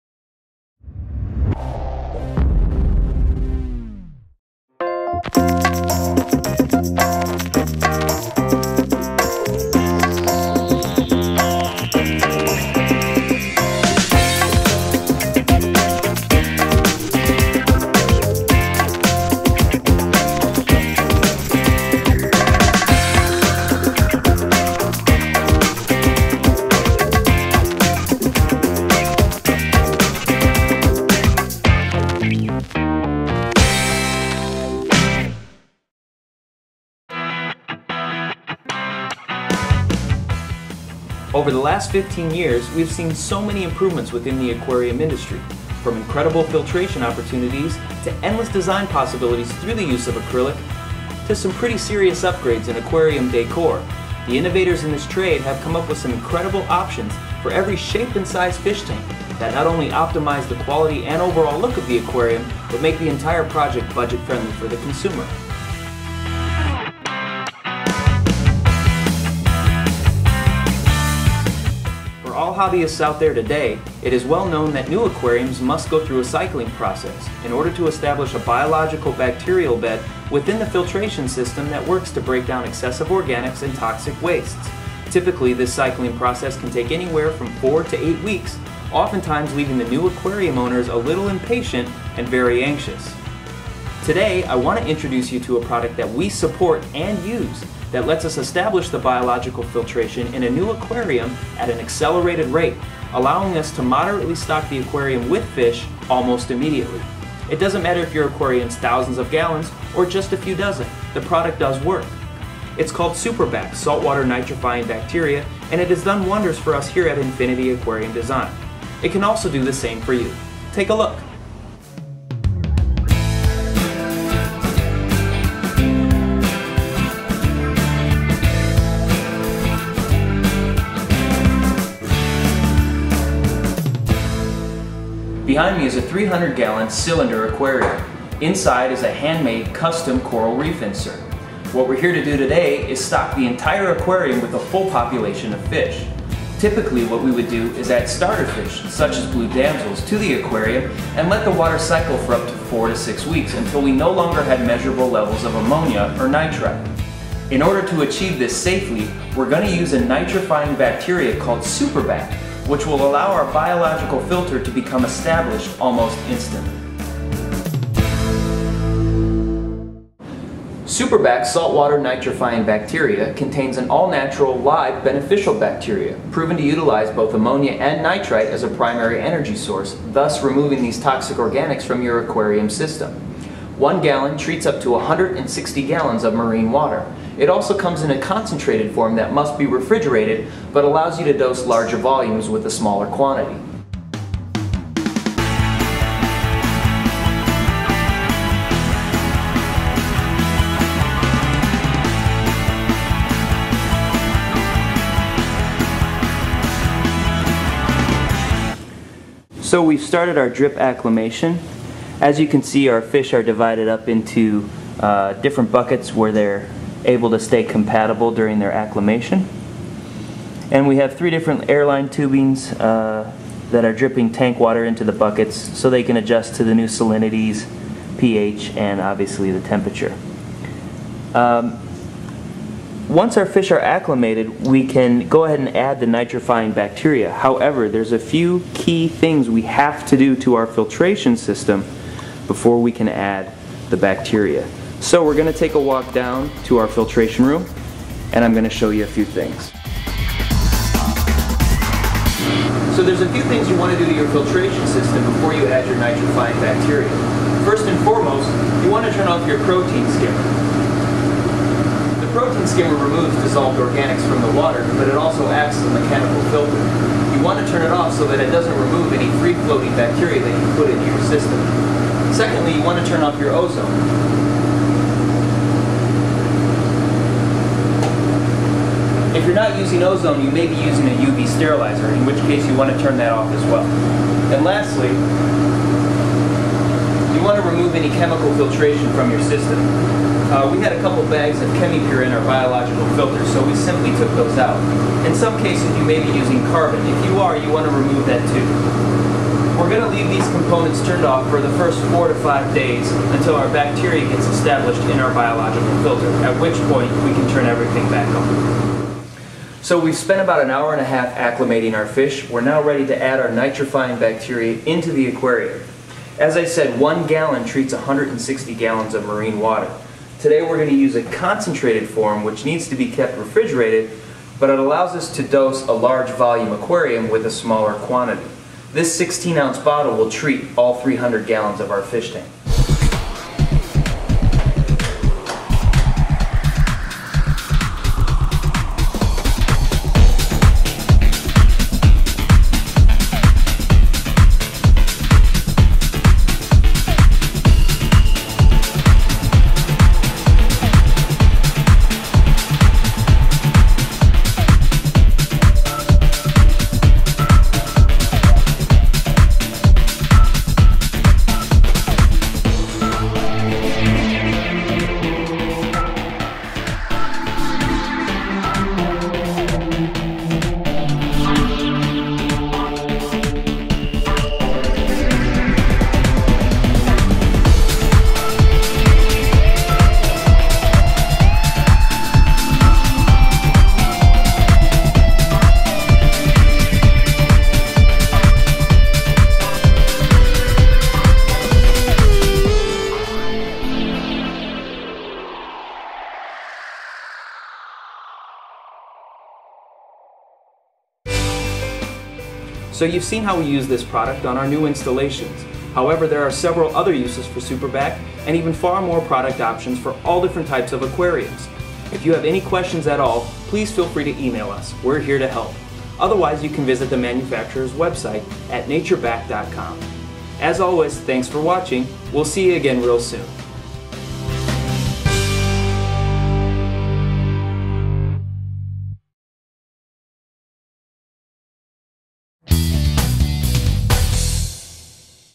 Custom Aquariums and Saltwater Fish Los Angeles
Custom aquarium installers of acrylic aquariums and saltwater fish tanks, Infinity Aquarium Design of Los Angeles, showcase Superbac Nitrifying Bacteria- a safe and faster alternative for cycling to marine aquariums.